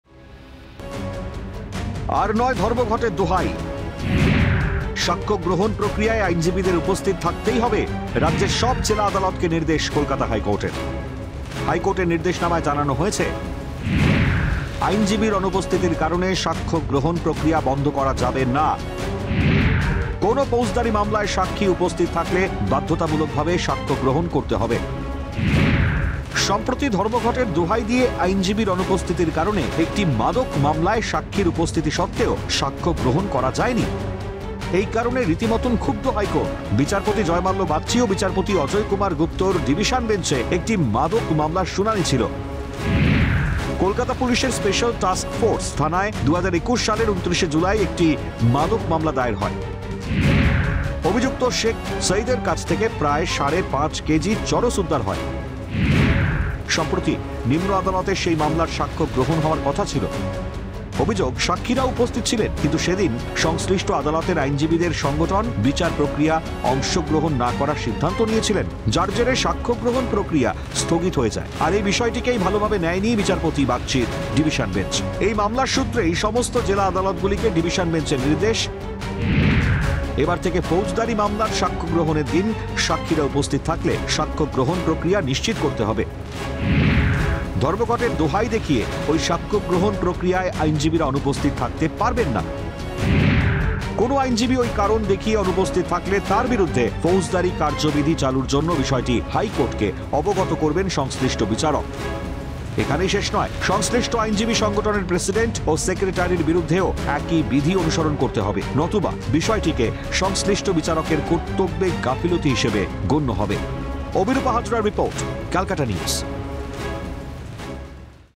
ধর্মঘটের দোহাই নয়, সাক্ষ্যগ্রহণে হাজিরা বাধ্যতামূলক
আর নয় ধর্মঘটের দোহাই। সাক্ষ্যগ্রহণ প্রক্রিয়ায় আইনজীবীদের উপস্থিত থাকতেই হবে, রাজ্যের সব  জেলা আদালতকে নির্দেশ কলকাতা হাইকোর্টের।